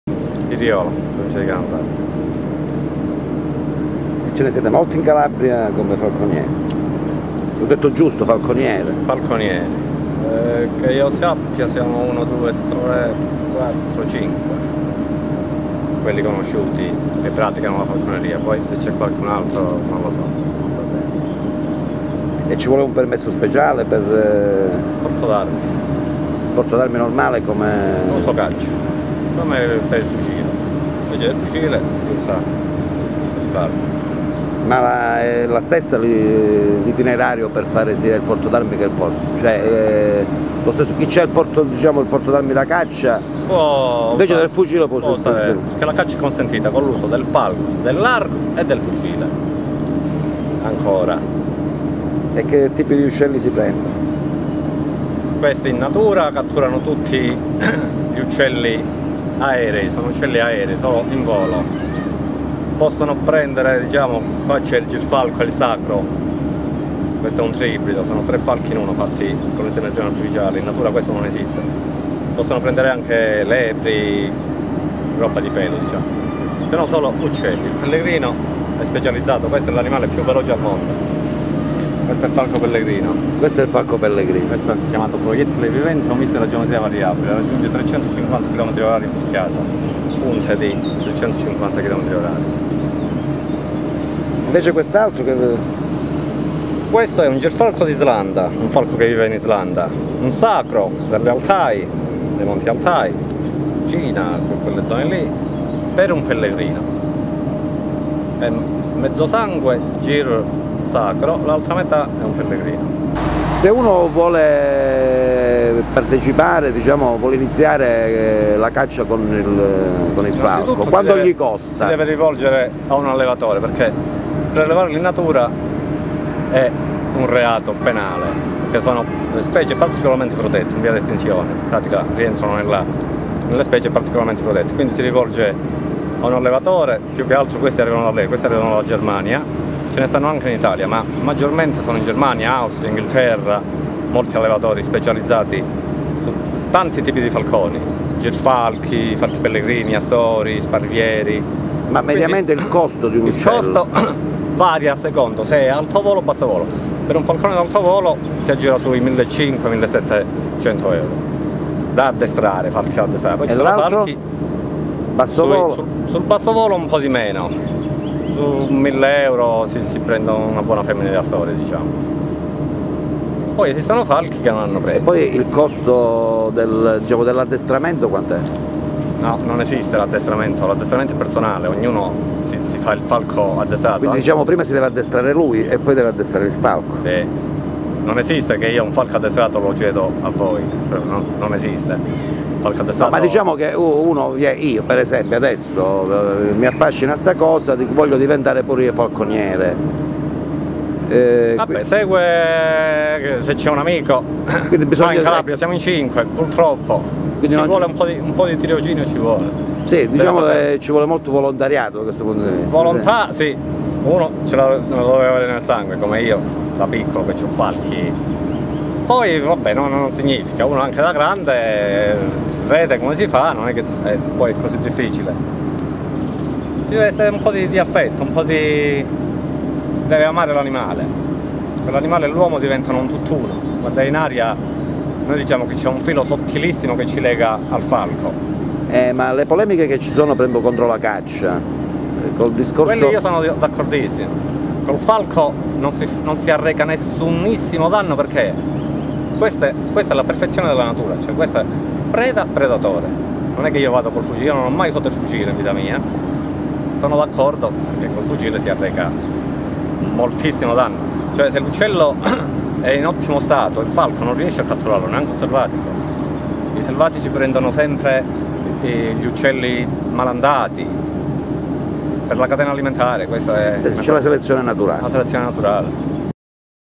falconeria
Cosenza giugno 2005, villa comunale, i falconieri ci presentano i loro campioni